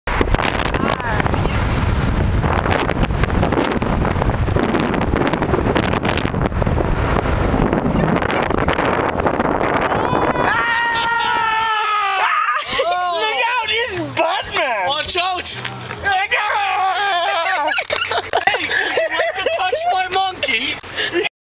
Batman vs Gorilla
Driving down the road and run into a Gorilla battling Batman in a duel...